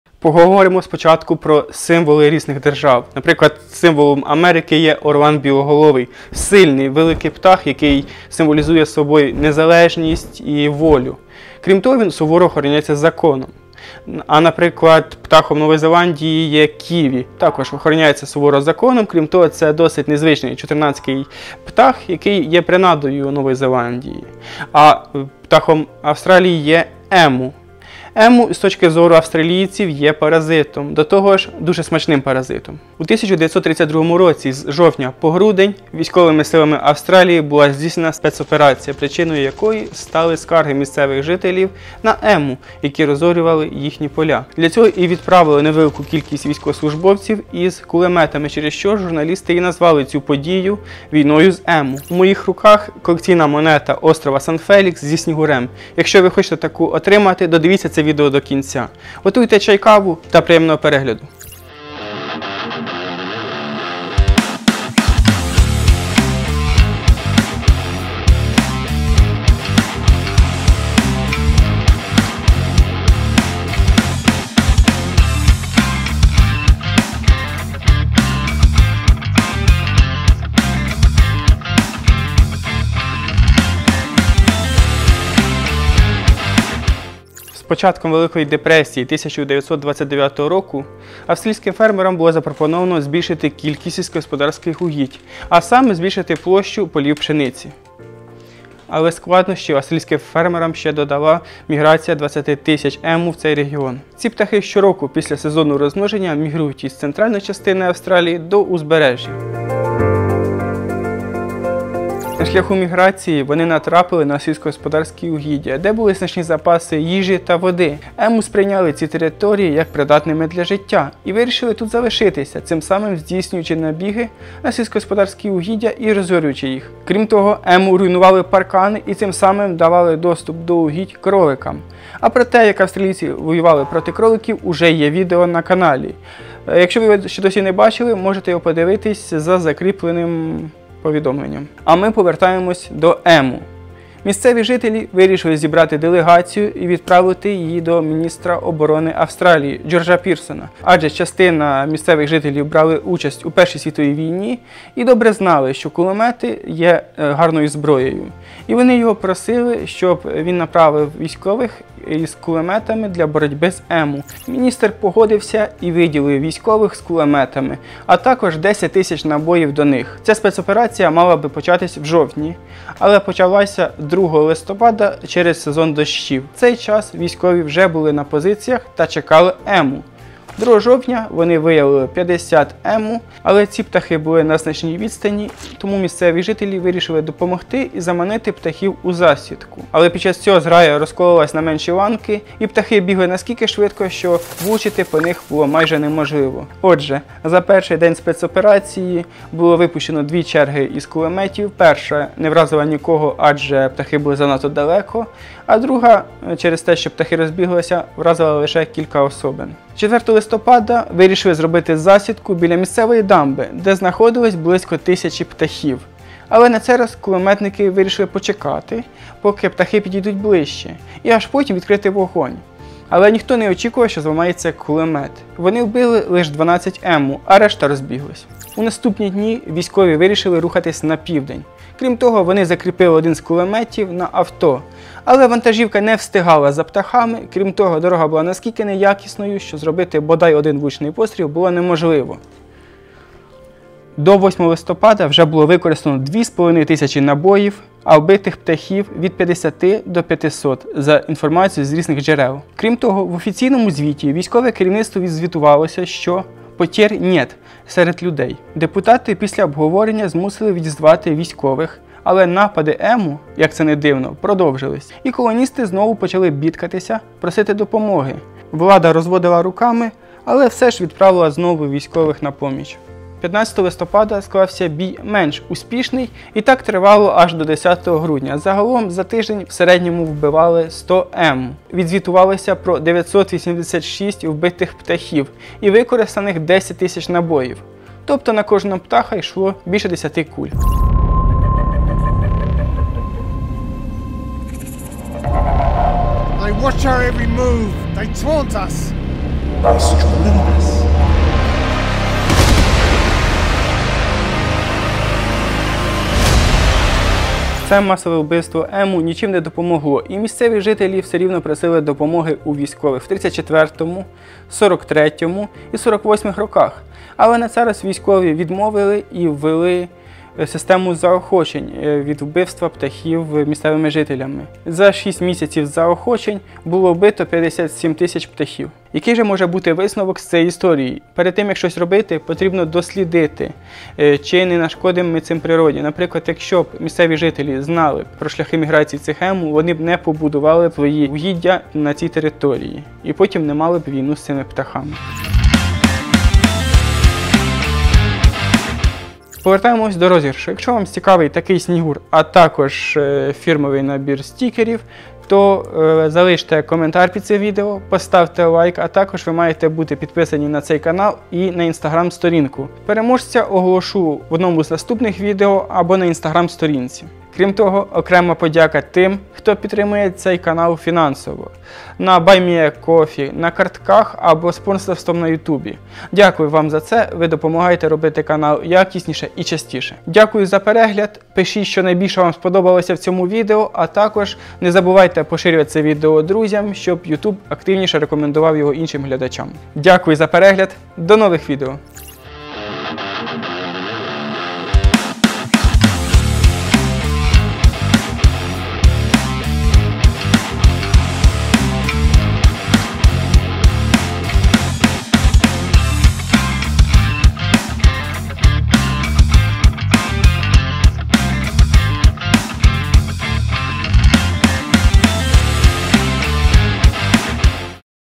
Війна з ему - військова спецоперація яка зазнала краху. Австралія та страуси ему.
У 1932 році з жовтня по грудень військовими силами Австралії була здійснена спецоперація, причиною стали скарги місцевих фермерів на страусів ему які знищували врожаї пшениці у західній Австралії. Для цього відправили невелику кількість військових із кулеметами через що журналісти й назвали цю подію “війною з Ему”.

00:00 Вступ
01:27 чому сталася "війна проти ему" ?
03:00 перший "бій" з ему
03:29 другий день
04:03 перший звіт до керівництва Австралії
04:32 друга фаза протистояння
05:07 підсумки цих подій
05:30 висновки
05:48 розіграш монети зі снігурем
06:09 подяка спонсорам проекта
✨Слідкуйте за нами:
 ✔️Станьте нашою пташкою🐦:
  😊Дякуємо за підтримку. Ви - чудові.